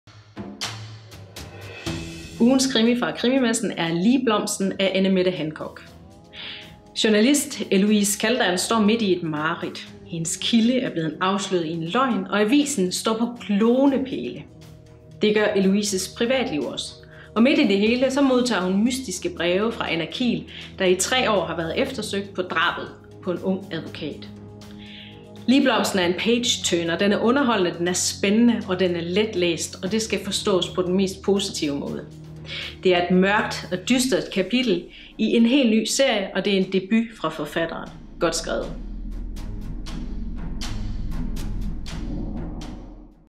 Ugens krimi - Ligblomsten
Lida har læst og anbefaler varmt Anne Mette Hancocks krimidebut "Ligblomsten".